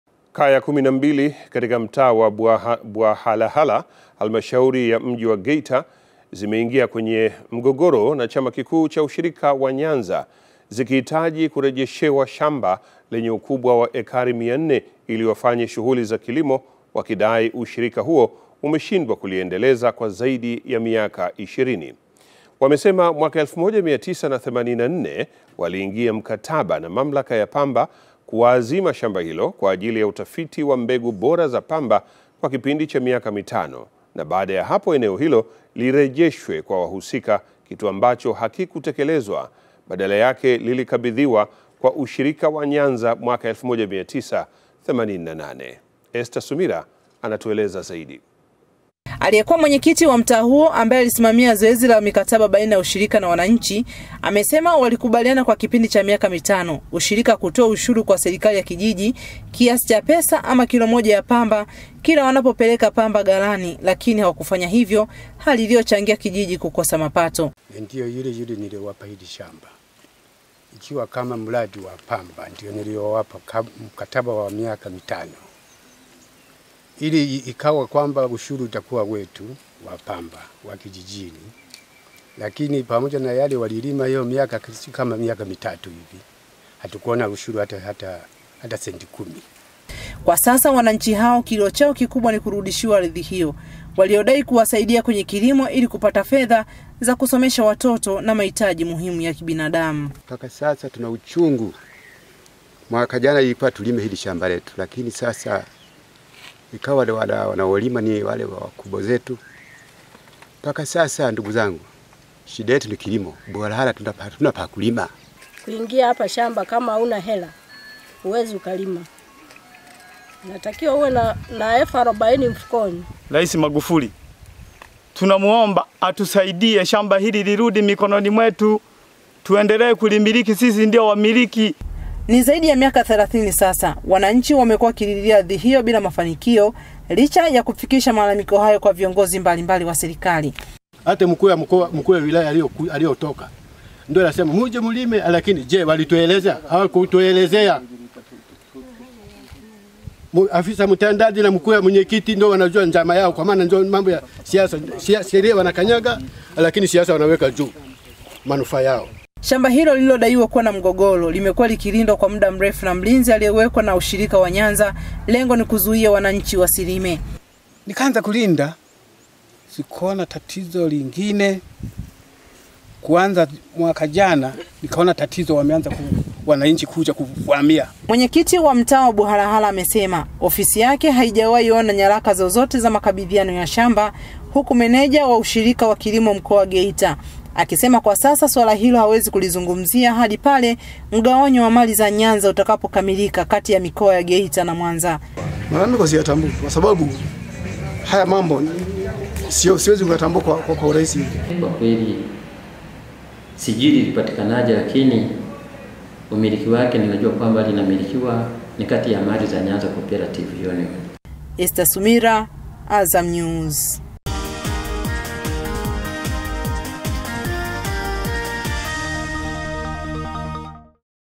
Familia 12, Ushirika wa Nyanza wavutana umiliki wa shamba la ekari 400
Kaya 12 mkoani Geita zimeingia kwenye mvutano zikitaka kupewa shamba la ekari 400 na Ushirika wa Nyanza ili ziweze kulima kwa ajili ya kuhudumia familia wakidai shamba hilo lilikabidhiwa kwa Ushirika huo kimakosa.